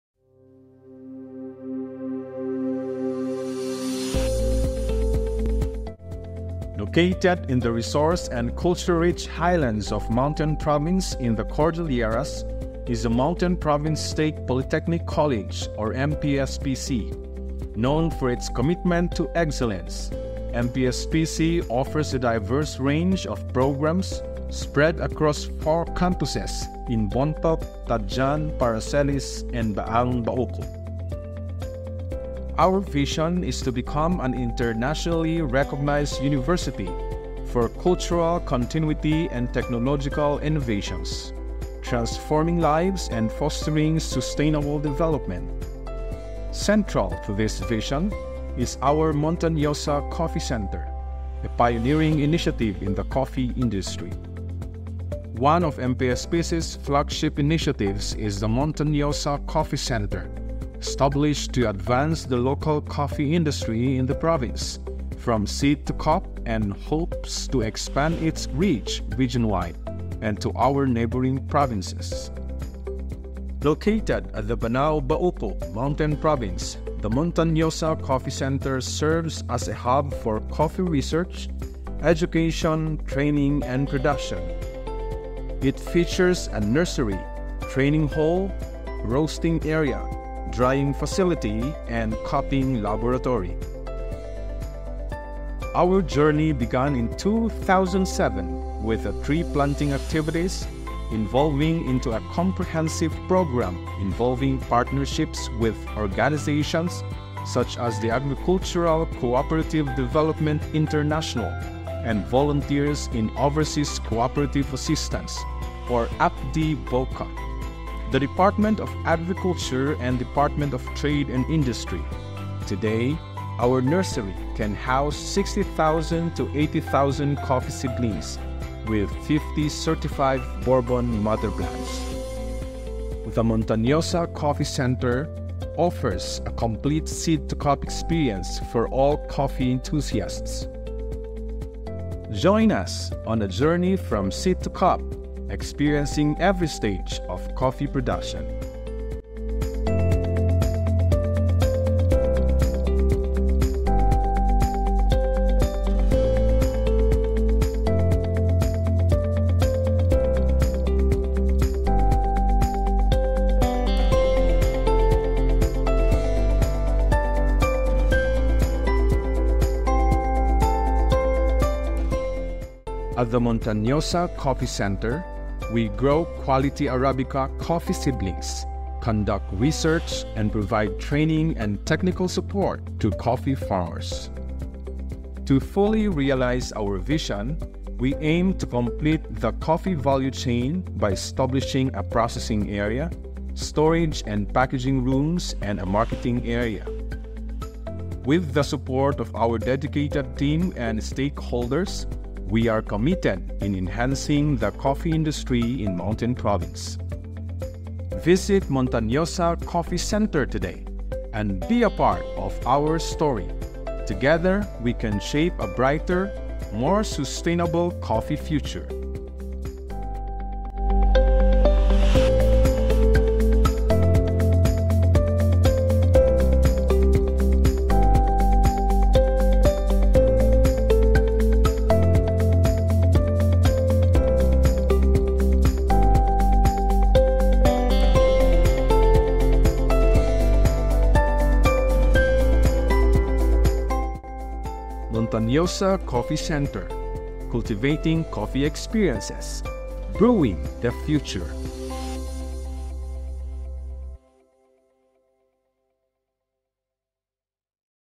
MPSU's Montañosa Coffee Center
The Montañosa Coffee Center is one of the flagship initiatives of the University. It is located at the University's Ba-ng campus in Bauko, Mountain Province.